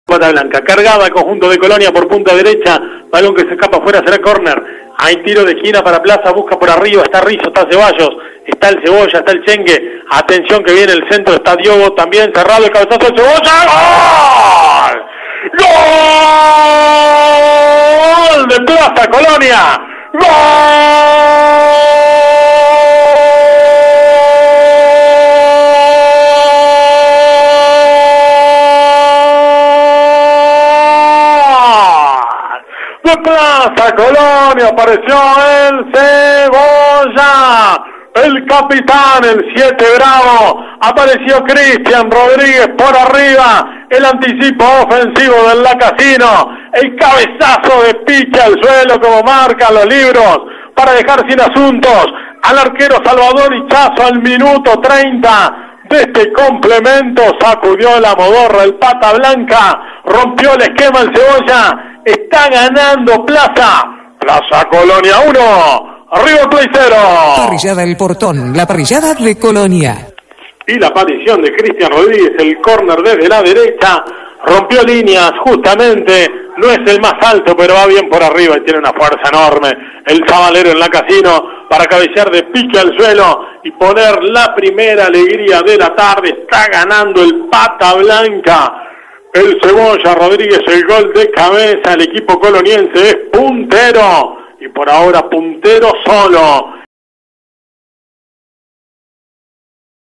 River Plate 0 Plaza Colonia 1 - Apertura 2021 (por Juan Prandi)
El gol de Cristian Rodríguez, por Juan Prandi para Amanecer FM 91.9 de Colonia.